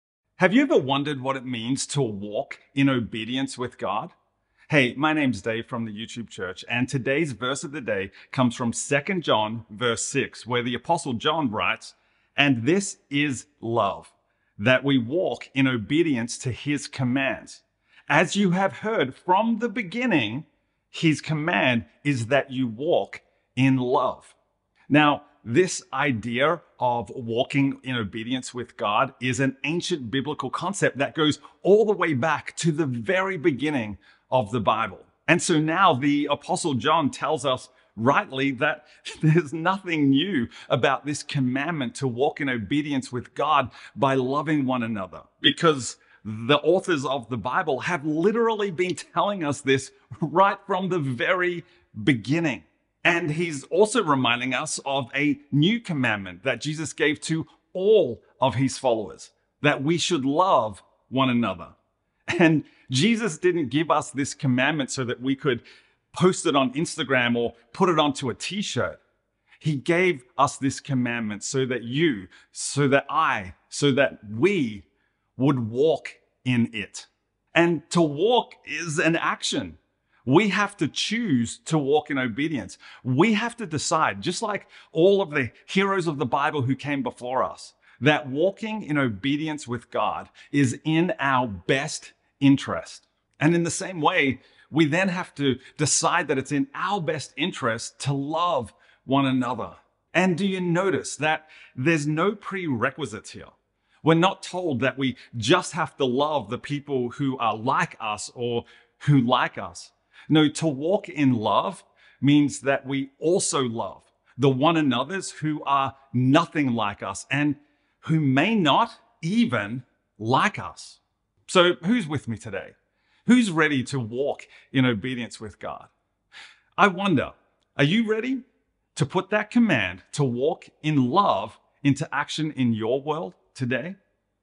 Walk in Love. 2 John 1:6
This video is The Verse of the Day on 6.17.24, extracted 2nd John Chapter 1 Verse 6, English Standard Version (ESV).

‭2 John 1:6 ESV‬
[6] And this is love, that we walk according to his commandments; this is the commandment, just as you have heard from the beginning, so that you should walk in it.